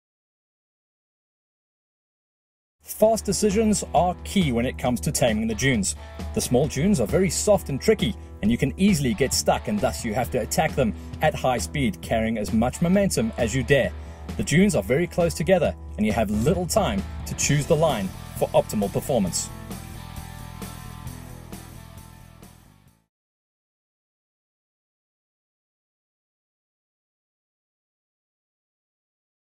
Dune training in the Nambian desert
Fast decisions are key when taming the dunes. Sand can be very soft and you have very little time to choose the line for optimal performance.